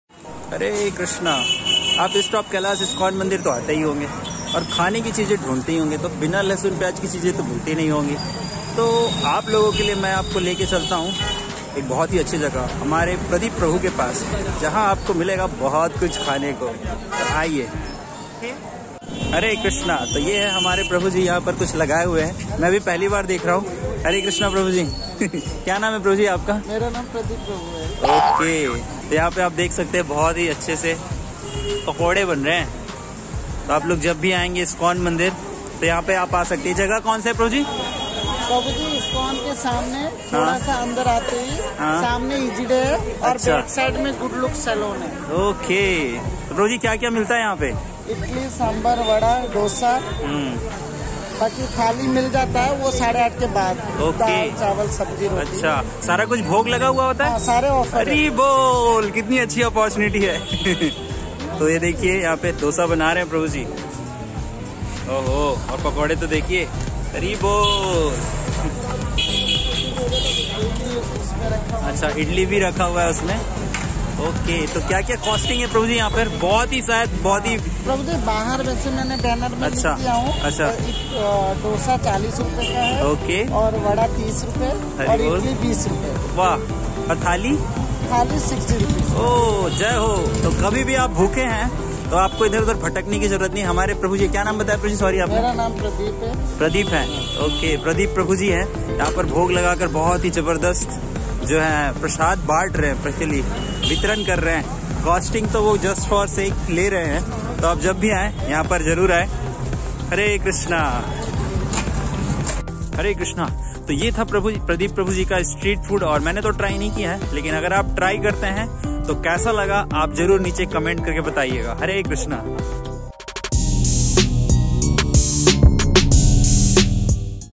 Good News | Street Food | Krishna Prasadam | Near EOK ISKCON Temple | Wow | Try It | Trending | Vlog
Here you will get offered Krishna prasadam outside the east of Kailash Temple of ISKCON Delhi